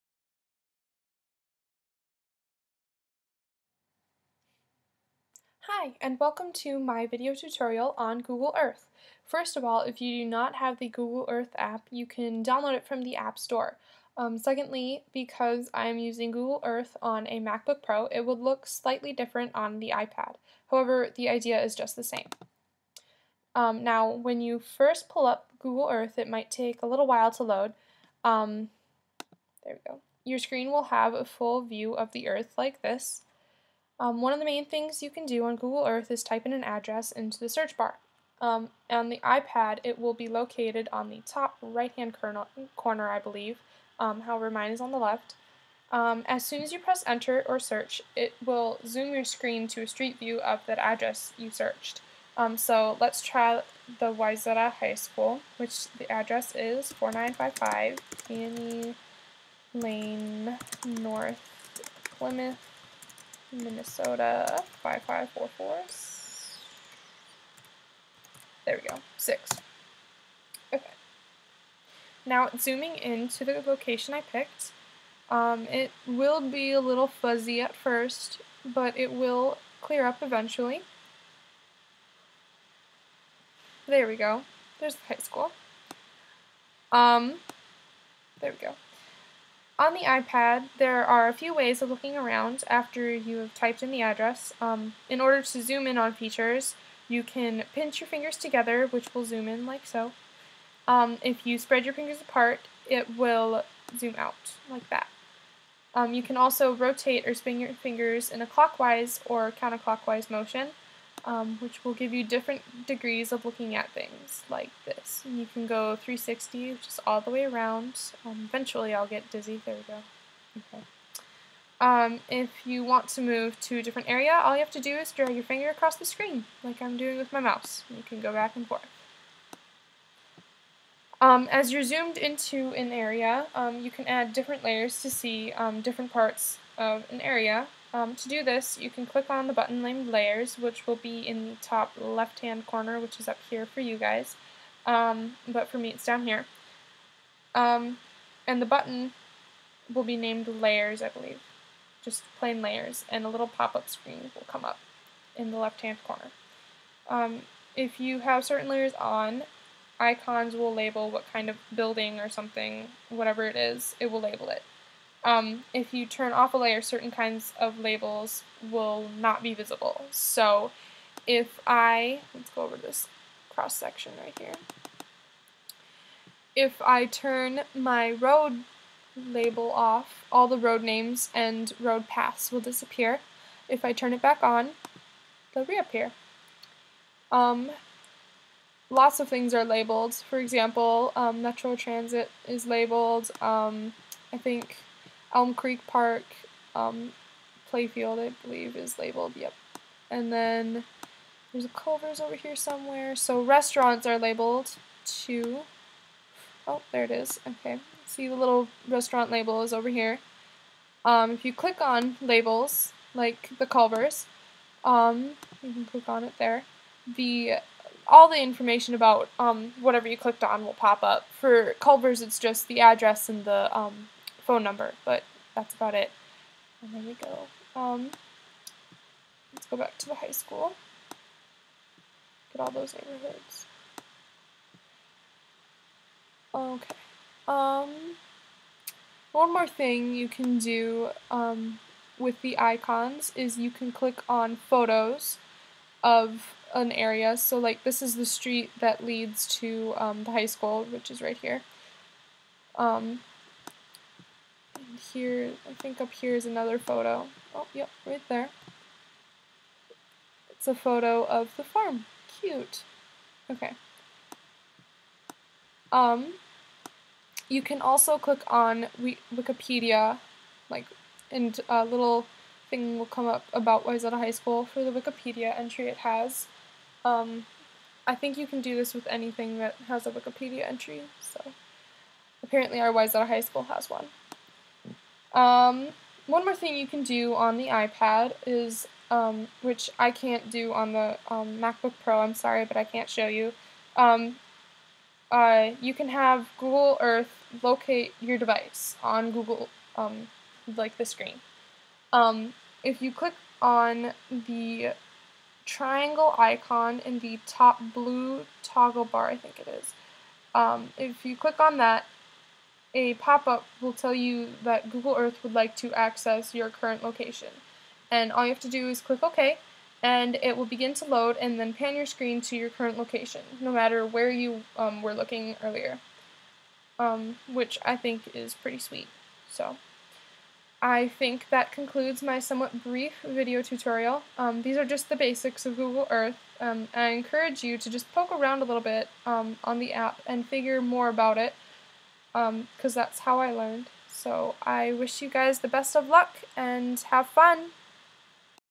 Google Earth Video Tutorial
A tutorial teaching google earth on a mac book pro.